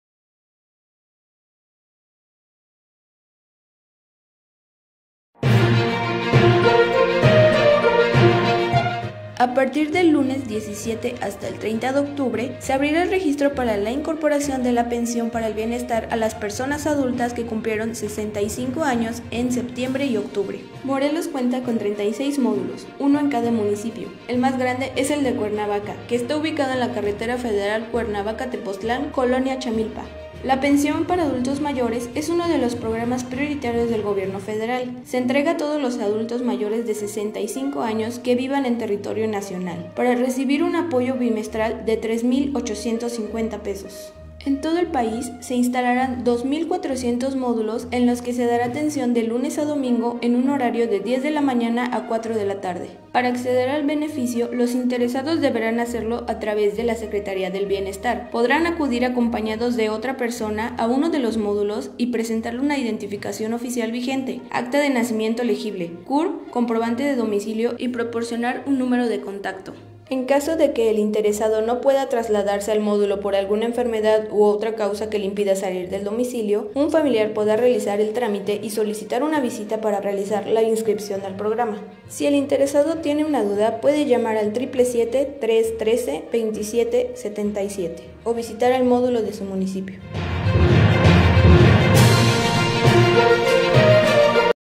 Abren hoy en Morelos registro para Pensión de Bienestar
Del 17 al 30 de octubre se abrirá el registro para la incorporación a la Pensión para el Bienestar de las Personas Adultas Mayores que cumplieron 65 años en septiembre y octubre; en cada municipio se instalará un módulo.